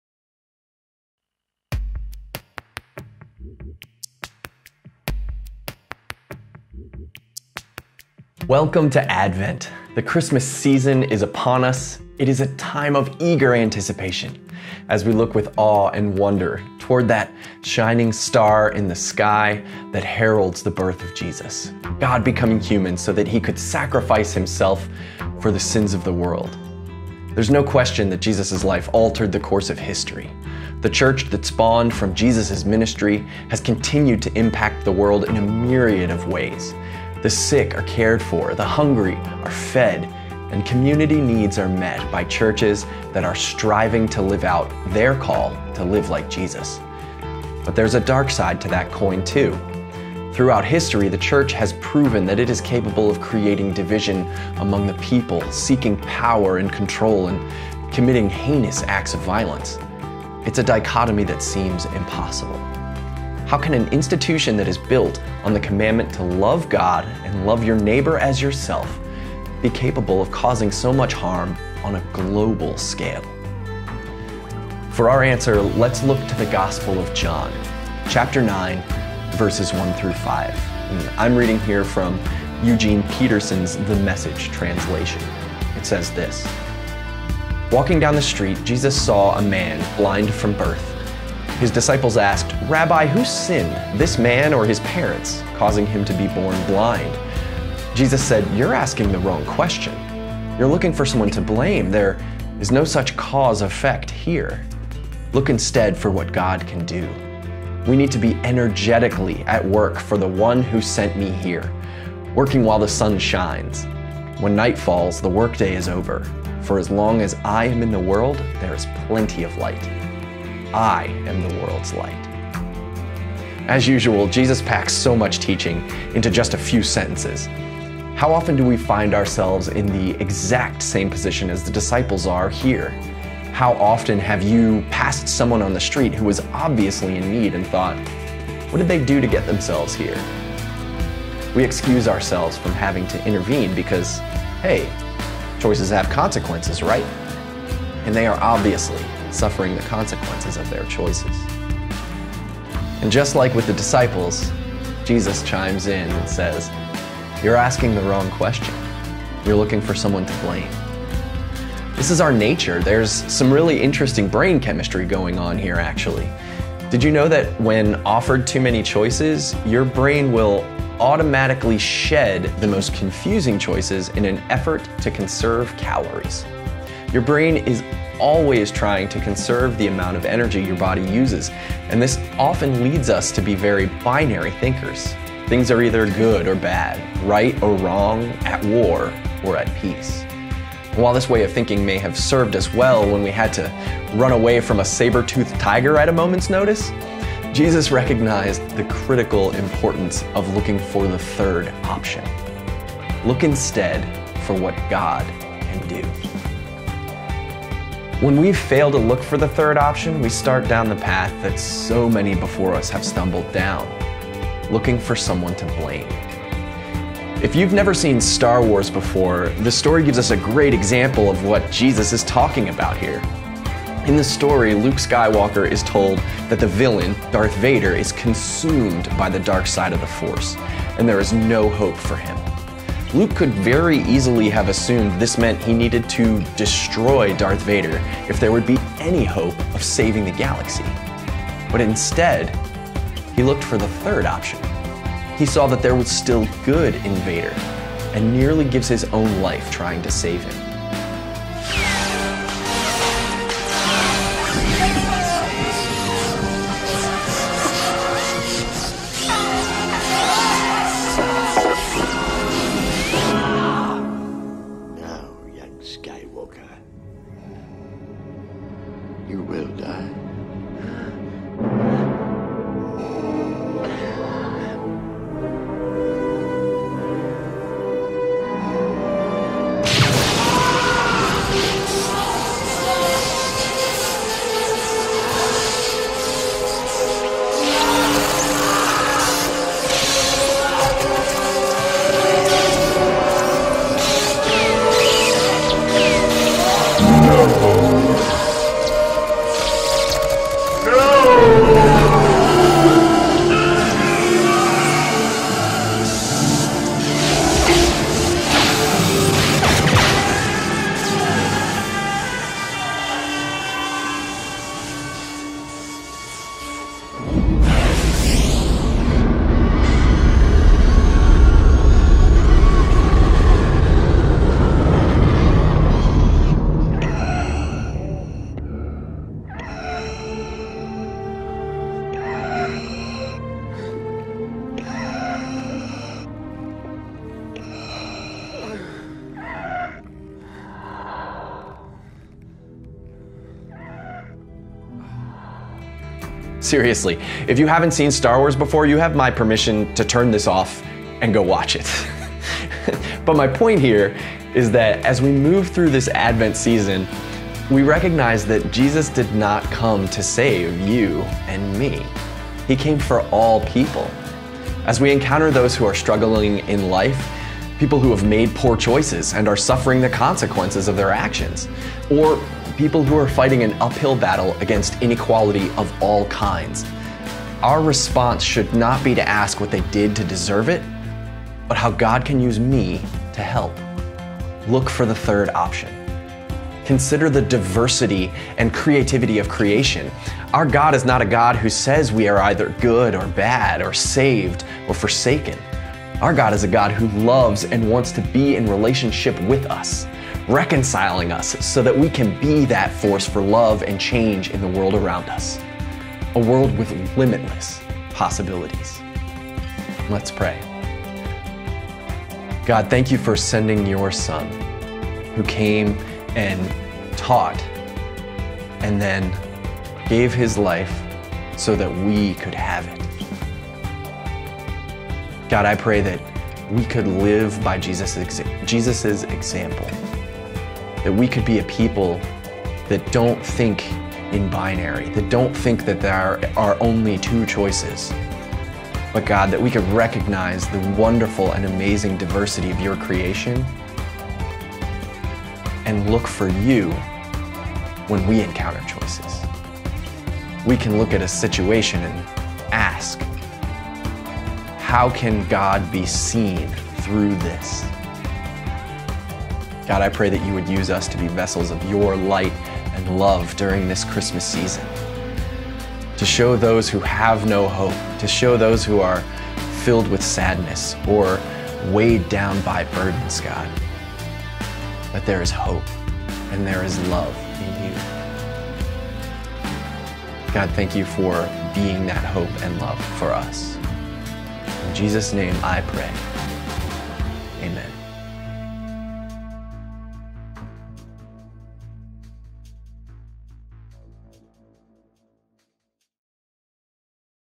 Common Table: Episode 027 - Advent - War & Peace Pt. 1
As we journey through Advent this year we'll spend some time think about the ways we are called to live in community with one another and how the arrival of Jesus on Christmas shapes the way we understand God.